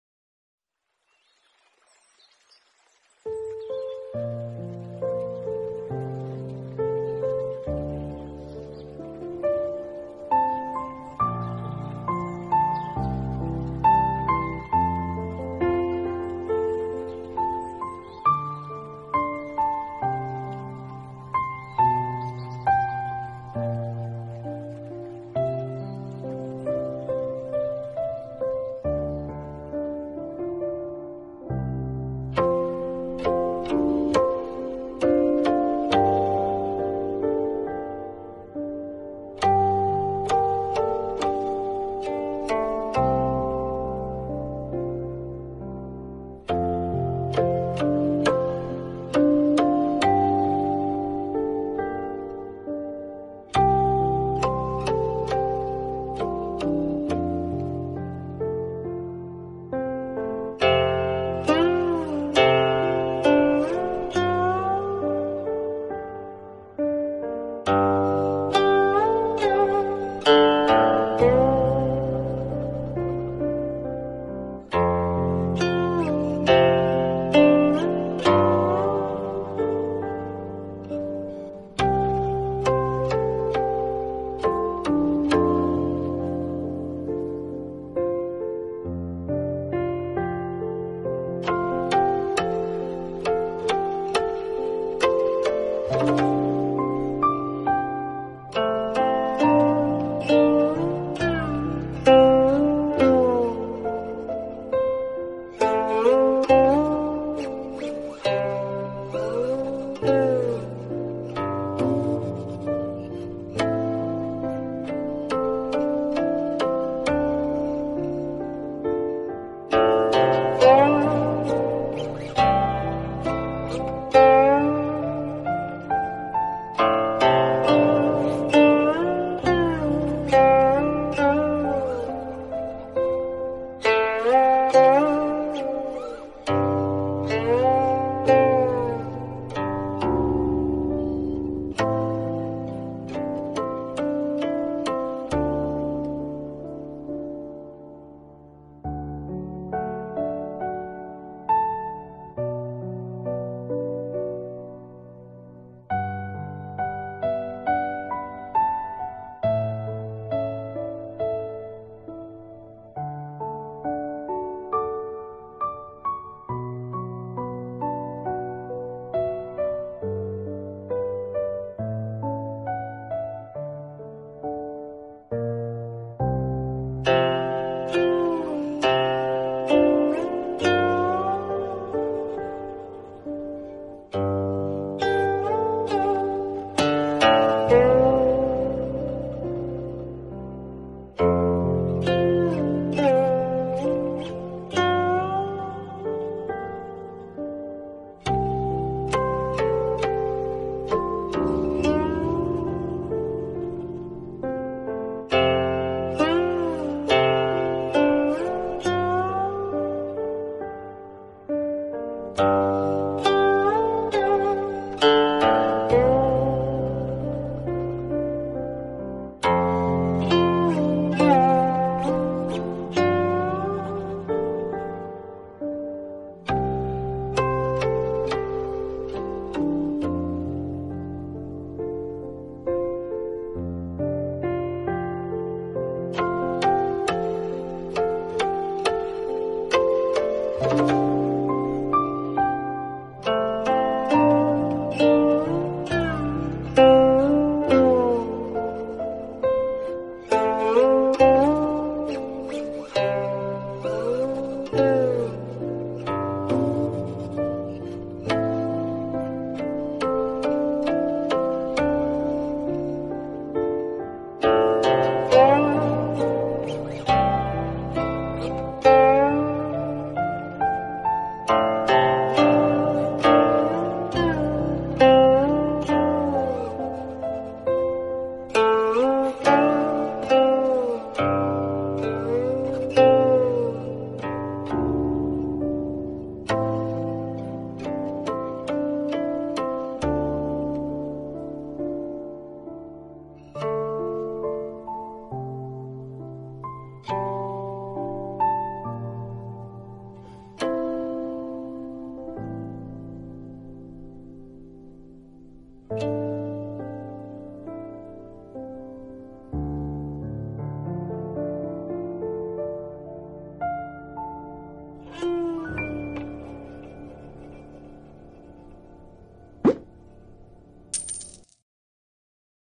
Hear to Cure | Spleen Nourishing with Palace Tones | Ideal for Overeating, Digestive Issues, and
Nourish your spleen with 'Hear to Cure'. Palace tones for those facing overeating, digestive issues, and a lack of luster. A healing listen.